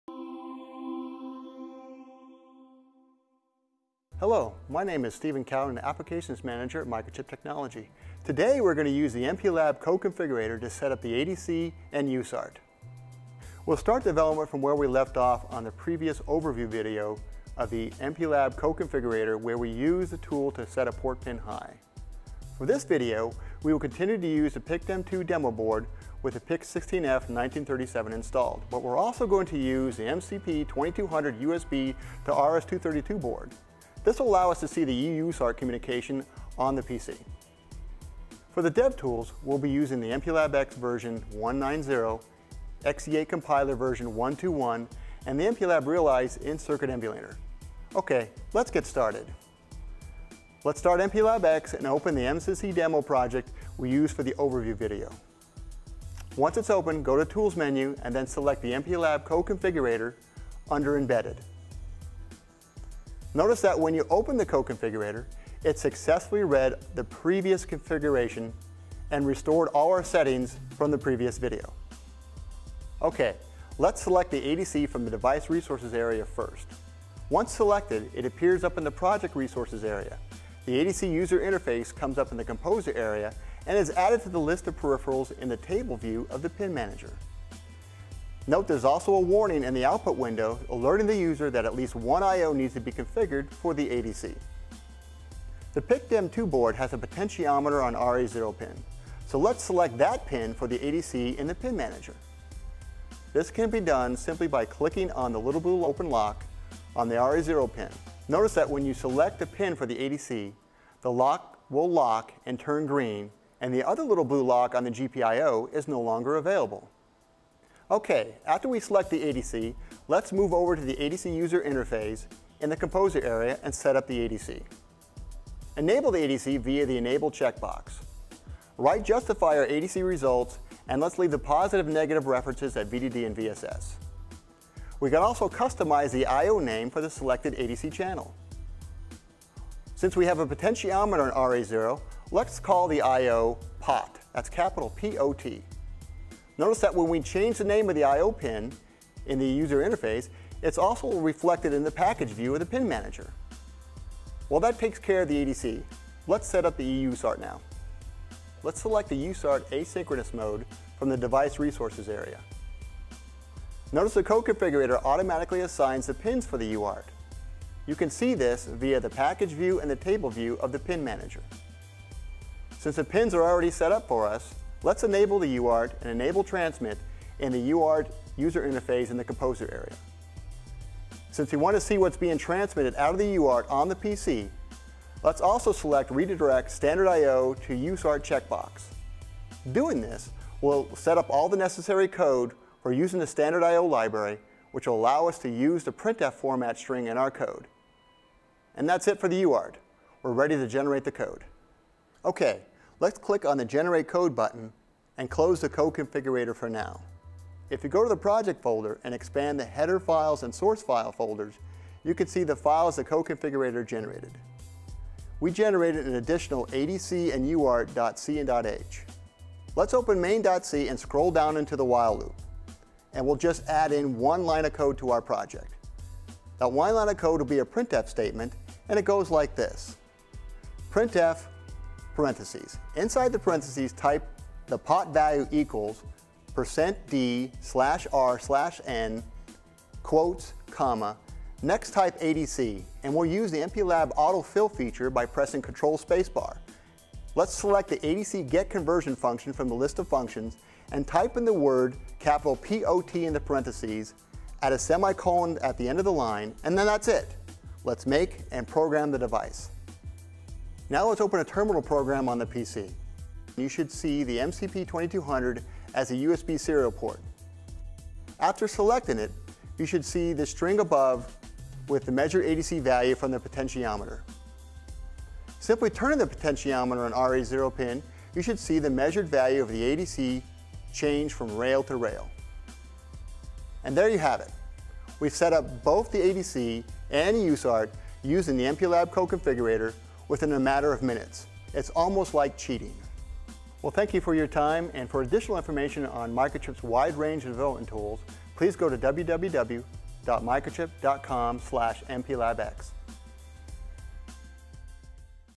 Using the MPLABX Code Configurator to setup the ADC and EUSART
Uses the MPLAB Code Configurator to generate both the initializers and driver functions for the ADC and EUSART.